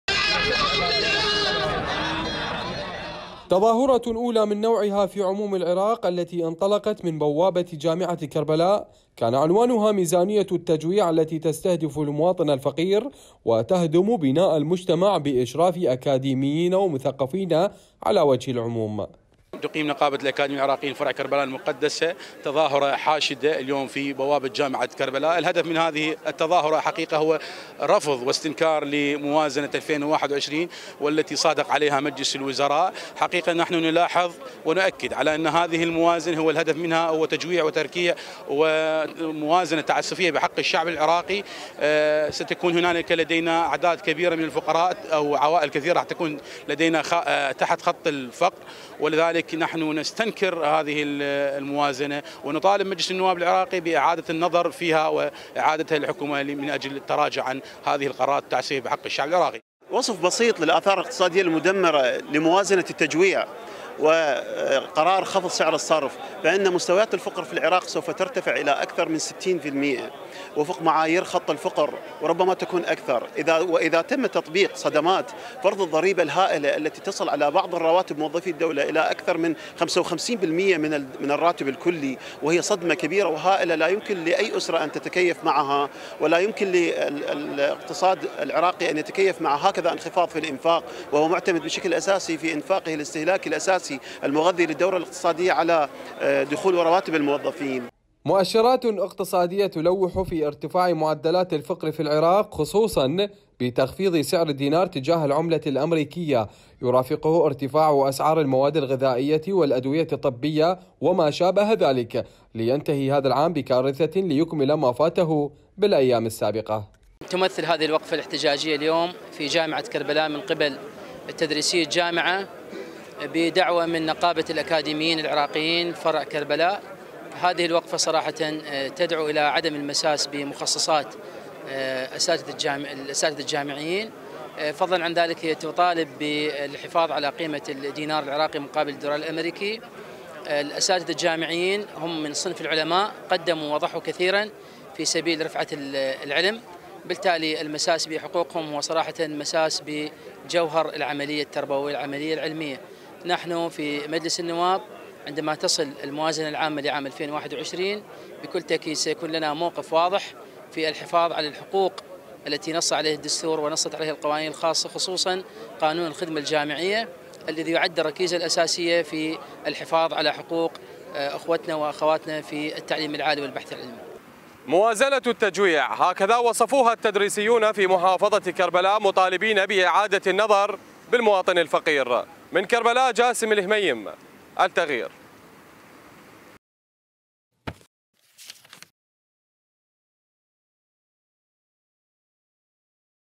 نتائج تظاهرة جامعة كربلاء الرافضة لموازنة التجويع | تقرير
نظم عدد من الأساتذة والأكاديميين في جامعة كربلاء وقفة احتجاجية ردا على بنود موازنة العام المقبل وتصريحات وزير المالية علي علاوي حول ضرورة استقطاع المخصصات من الأكاديميين وذلك ضمن حزمة إصلاحات حكومية لمعالجة الأزمة المالية